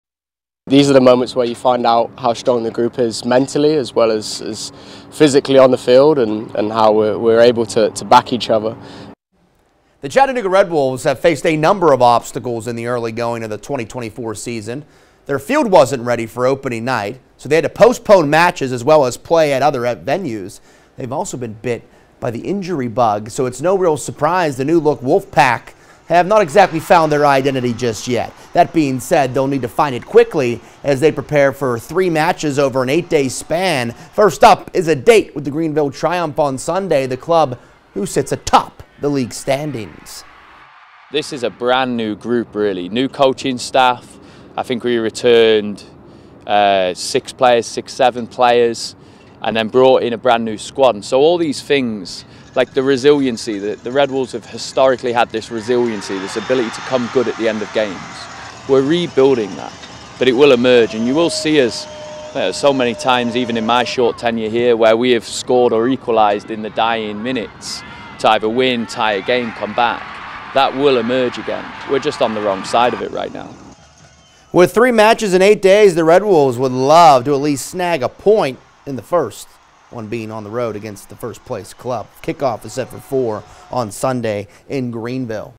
Red Wolves looking to find identity ahead of tough stretch
Chattanooga will play three matches in an eight-day span that includes a date with the first-place Greenville Triumph on Sunday at 4 p.m.